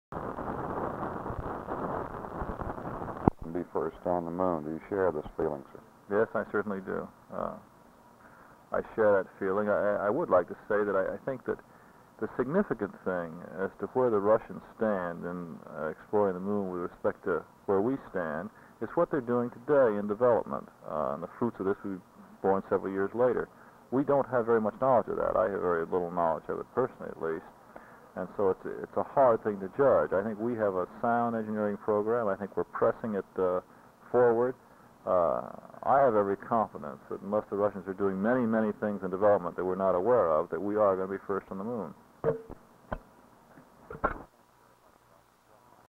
08/17/62 Director of Space Flight D. Brainerd Holmes speaks about who will be first to land on ...
4354
U. S. Director of Space Flight Brainerd D. Holmes was interviewed in Roanoke last night, after his address at the Lunar conference at Virginia Tech in Blacksburg.
Holmes told N Channel 10 news and Public Affairs Director Joe Moffatt that he shared the feeling of many scientists that the U. S. will be first on the moon, regardless of the Russian space advances.
(take film. sound up. out cue: "Russians are doing many, many things that were not aware of, were going to be first on the moon.")